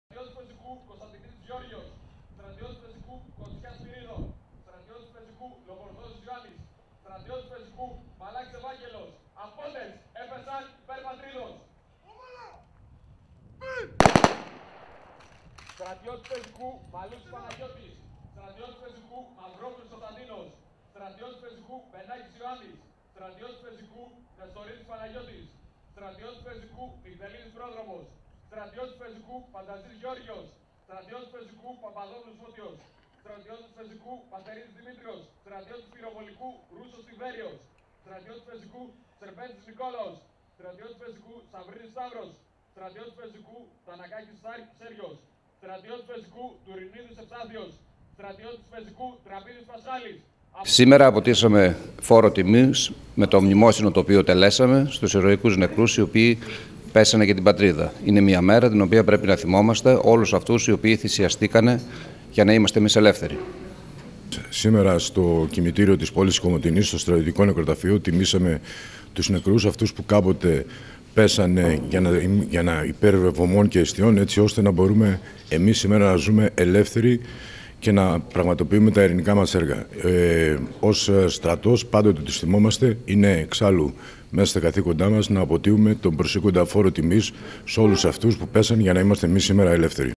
thrakionline.gr : Τρισάγιο το ψυχοσάββατο στην μνήμη πεσόντων κατά τους αγώνες του έθνους
thrakionline.gr : Το Σάββατο 22 Φεβρουαρίου 2020, πρώτο μεγάλο ψυχοσάββατο του έτους τελέστηκε ετήσιο μνημόσυνο υπέρ της ανάπαυσης των ψυχών των πεσόντων κατά τους αγώνες του έθνους στο στρατιωτικό κοιμητήριο της Κομοτηνής. Στο μνημόσυνο παραβρέθηκαν και οι δύο ταξίαρχοι της Ροδόπης, Λεωνίδας Γιοβάνης και Ιωάννης Μπόγδος όπου και μίλησαν για το μνημόσυνο αυτό.